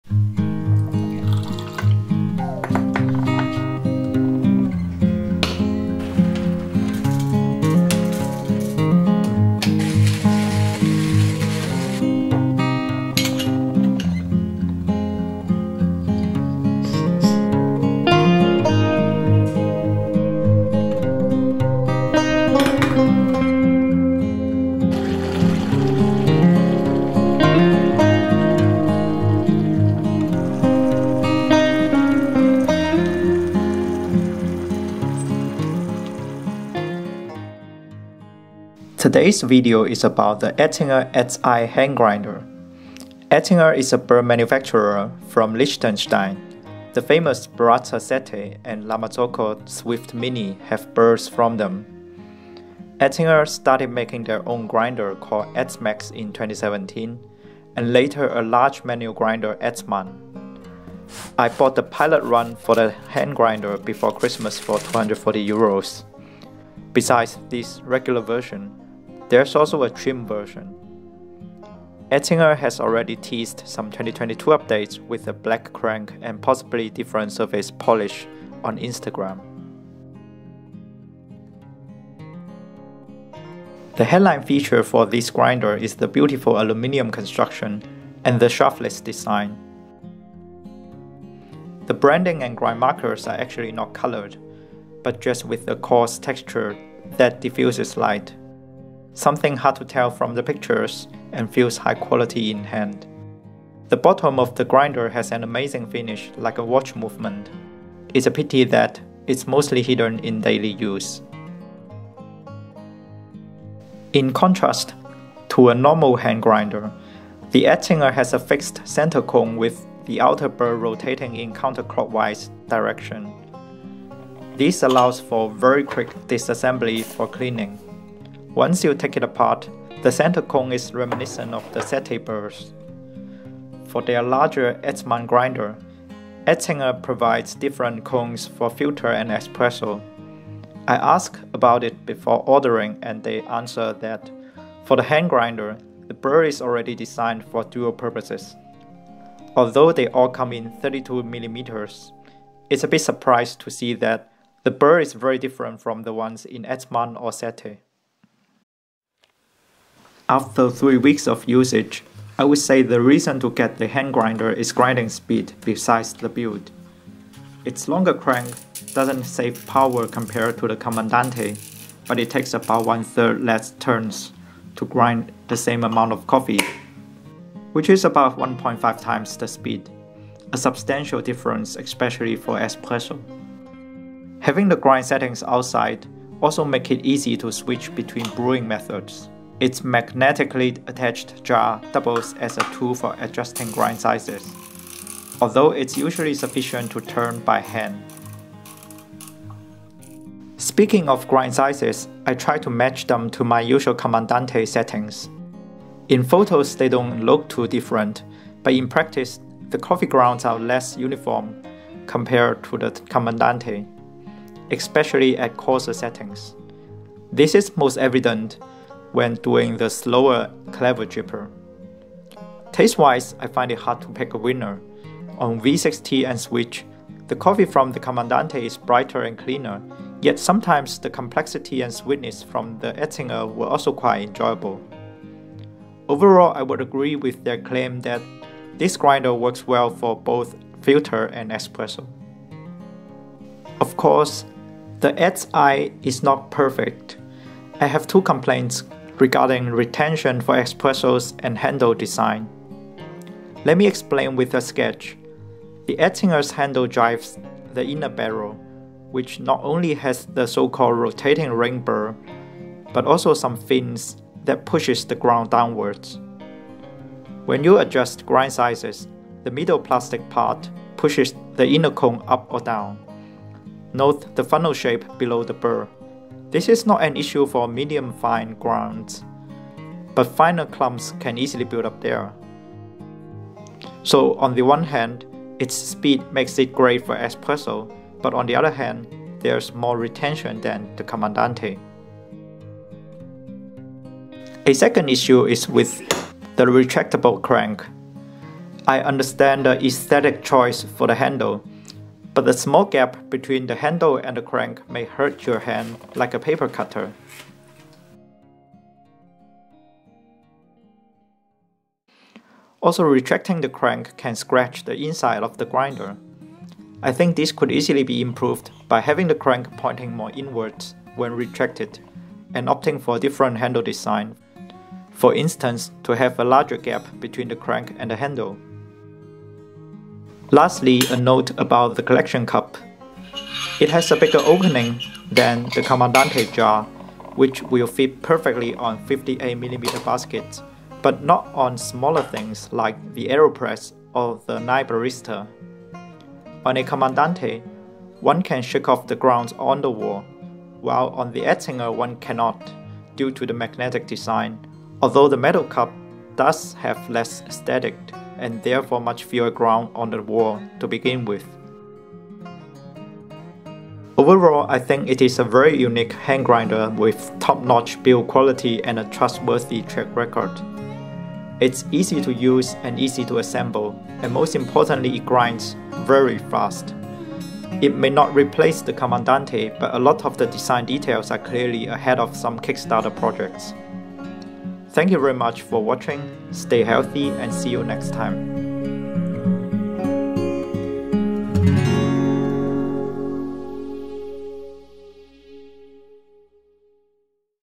Etzinger etz-I: A Fast Beautiful Grinder with Room to Improve
This is a quick review of the Etzinger etz-I hand grinder featuring aluminium body and a unique shaftless design. (Update: the funnel shape that retains some coffee grounds is confirmed to be Etzinger's Antistatic-Jig compressing the grounds to reduce static).

- Pros: It grinds very fast, about 1/3 fewer turns than Comandante C40, and is very easy to disassemble. The resulting coffee tastes good to me. Low retention or static for pour-overs.
- Cons: About 1g retention for Espresso grounds, the handle may pinch your hand.
- Note: The magnet in the collection cup will attract metal spoons.

You can also see the retention issue from Lance Hedrick's video on the etzMan at around 3:20 when he talks about different cones:

0:00 Prelude: an Espresso
0:38 Intro: Etzinger and etz-i
1:25 Build and burr
3:21 Ground comparison with Comandante C40
4:03 The issues with retention and handle
6:03 Conclusion